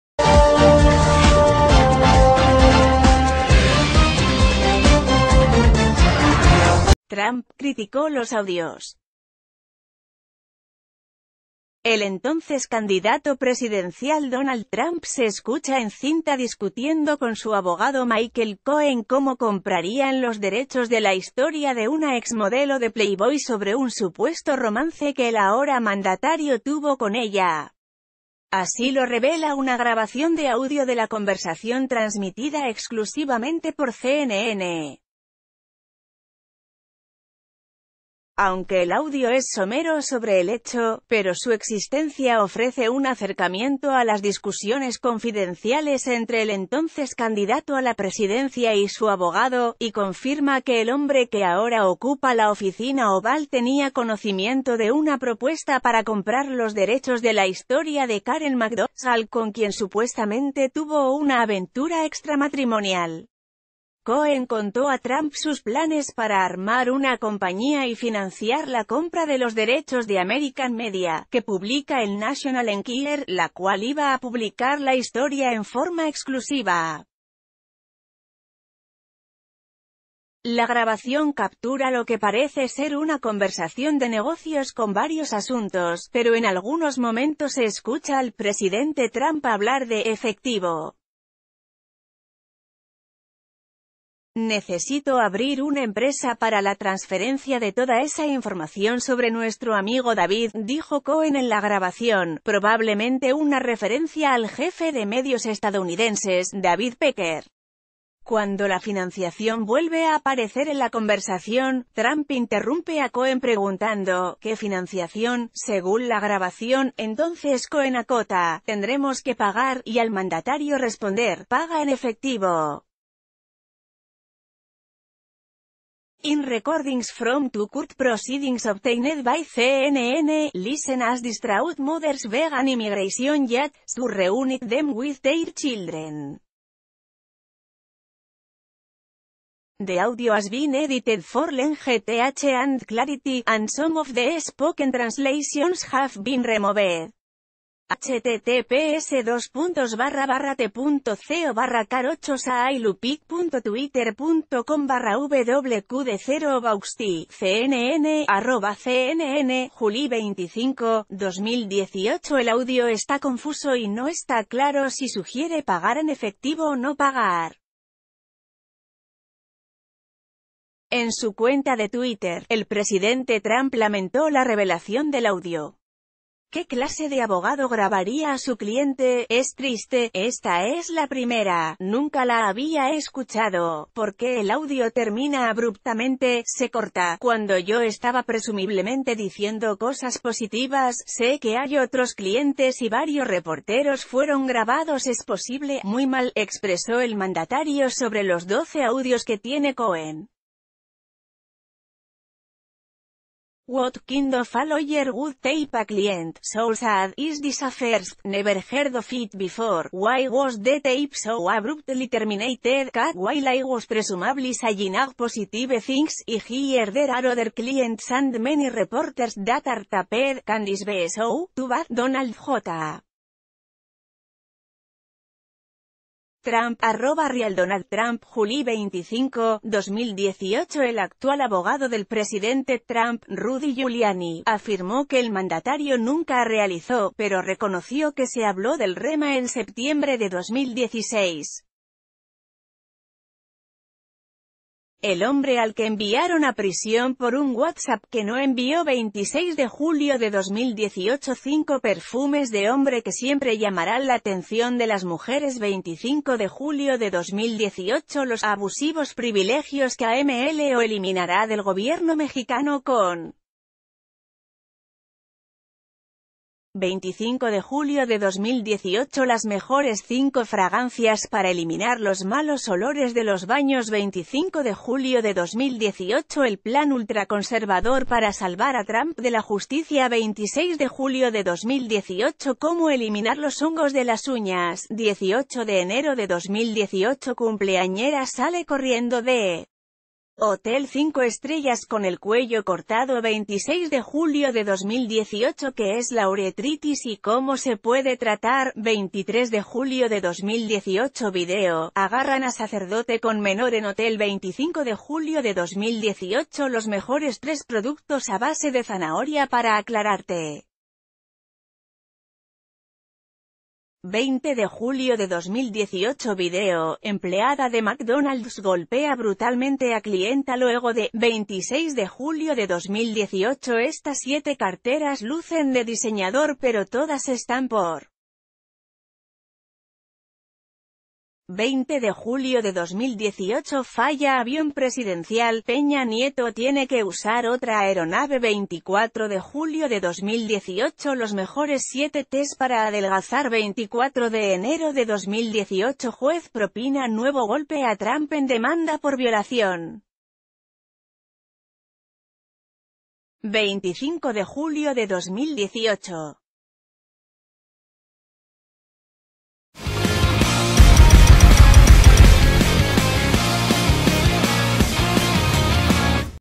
El polémico audio donde Trump habla de pagar a exmodelo de Playboy
Trump criticó los audios. El entonces candidato presidencial Donald Trump se escucha en cinta discutiendo con su abogado Michael Cohen cómo comprarían los derechos de la historia de una exmodelo de Playboy sobre un supuesto romance que el ahora mandatario tuvo con ella. Así lo revela una grabación d...